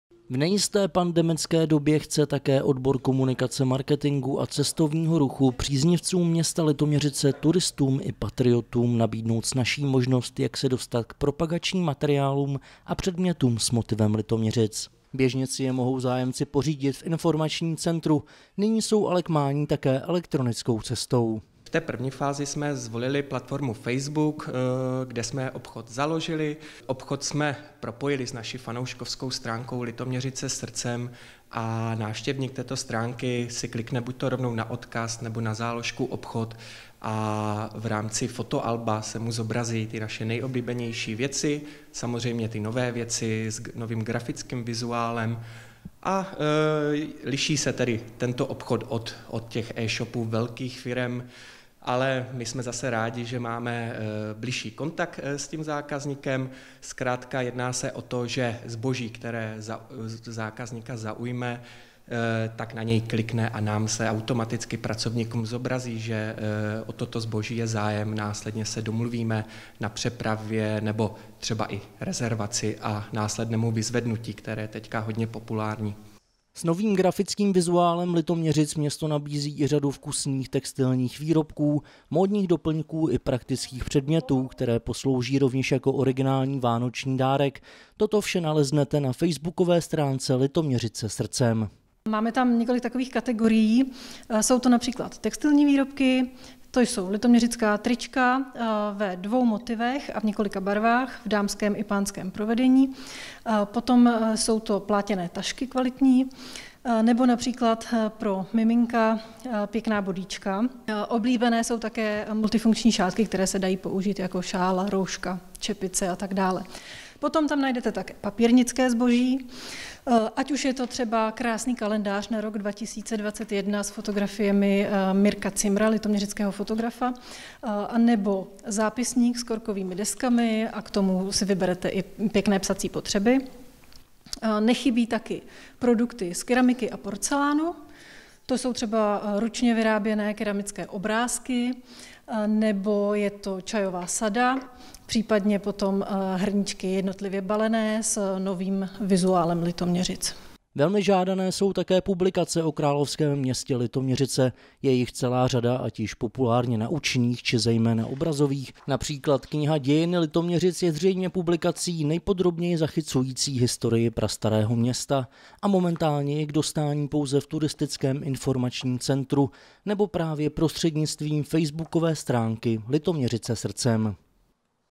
Vánoční dárky s motivem Litoměřic pořídíte přes internet
V nejisté pandemické době chce také Odbor komunikace, marketingu a cestovního ruchu příznivcům města Litoměřice, turistům i patriotům snažší možnost, jak se dostat k propagačním materiálům a předmětům s motivem Litoměřic. Běžně si je mohou zájemci pořídit v Informačním centru, nyní jsou ale k mání také elektronickou cestou.